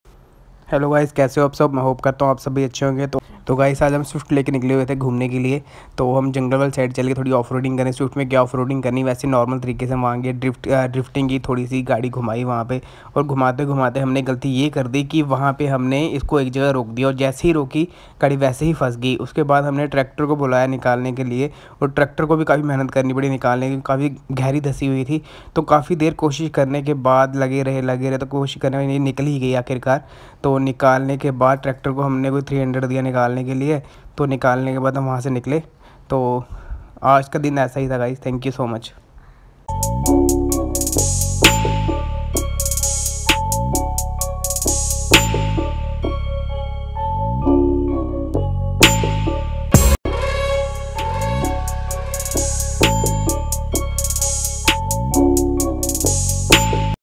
Off-roading gone wrong on swift 🥲
Idealist parshant
Blogs

This is short video how we recover our car from sand